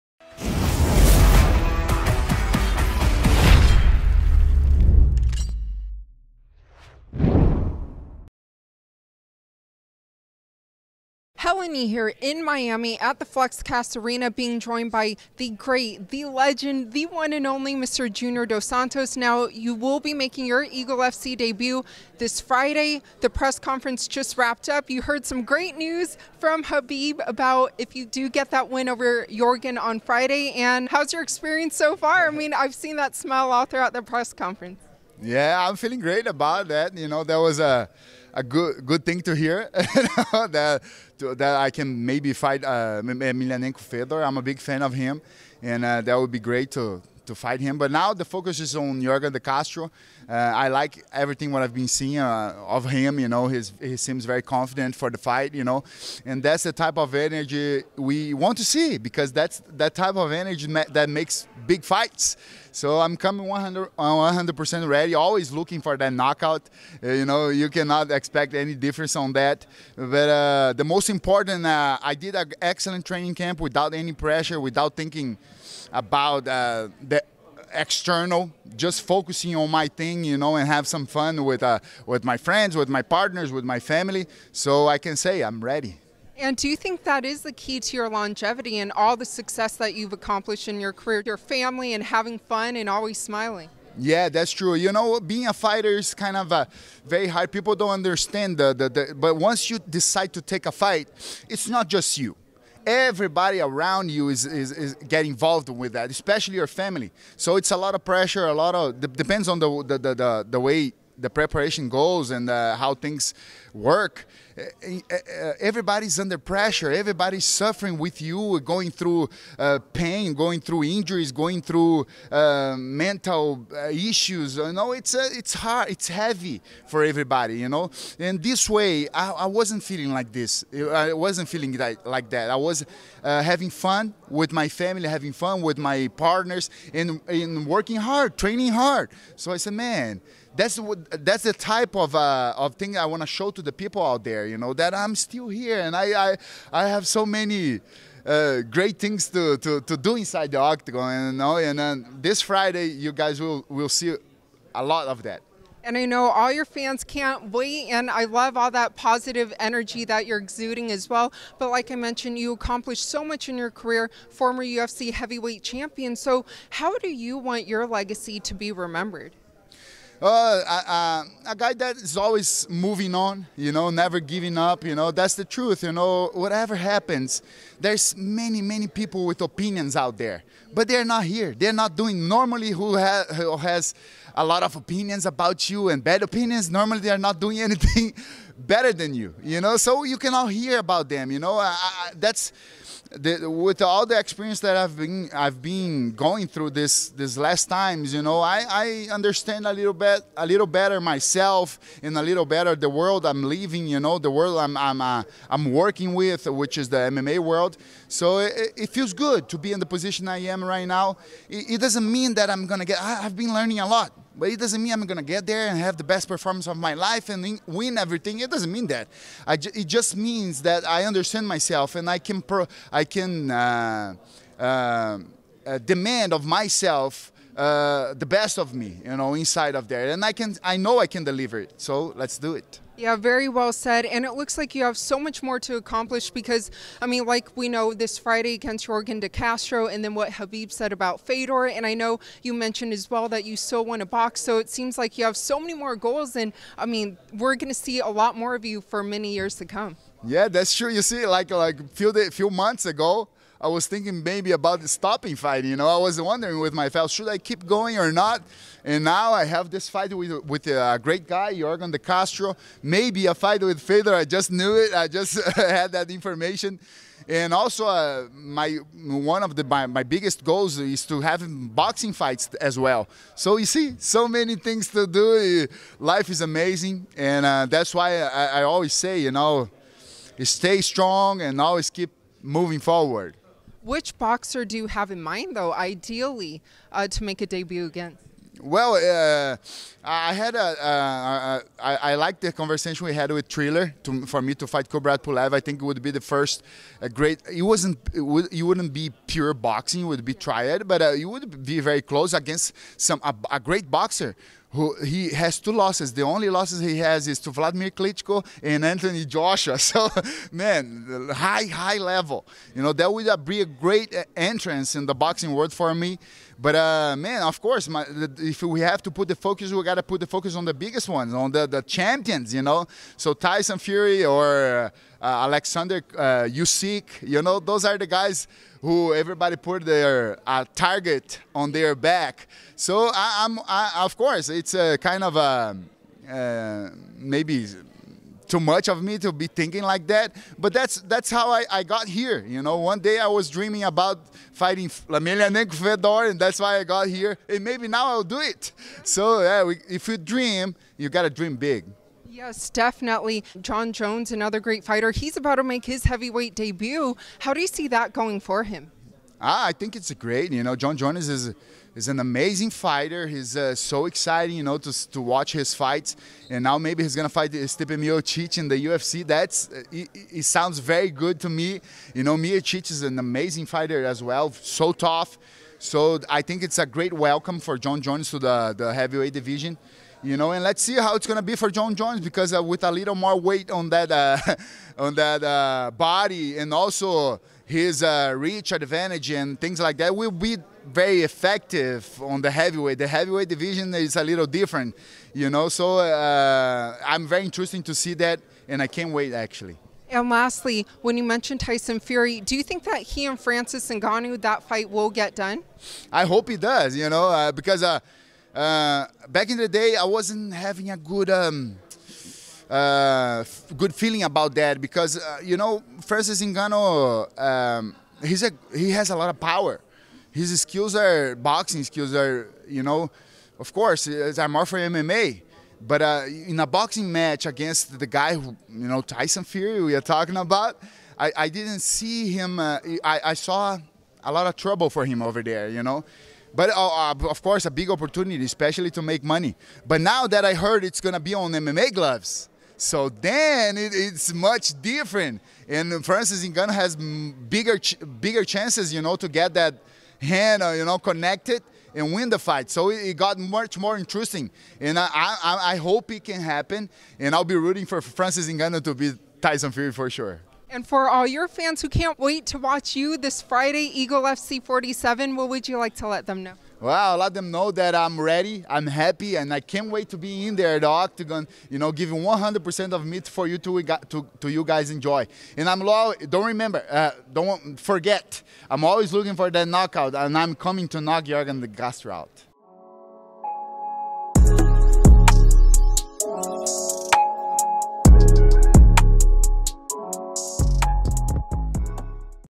Junior Dos Santos Candid On Francis Ngannou vs Tyson Fury, Jon Jones To Heavyweight
At Eagle FC 47 fight week, Junior Dos Santos spoke to Helen Yee about his upcoming fight against Yorgan De Castro, Khabib Nurmagomedov saying he will fight Fedor next if he beats Yorgan De Castro, what he credits to his success and longevity in his career, how he wants his legacy to be remembered, his goals, who he wants to box in his future boxing debut, Jon Jones heavyweight debut, Francis Ngannou vs Tyson Fury, and more.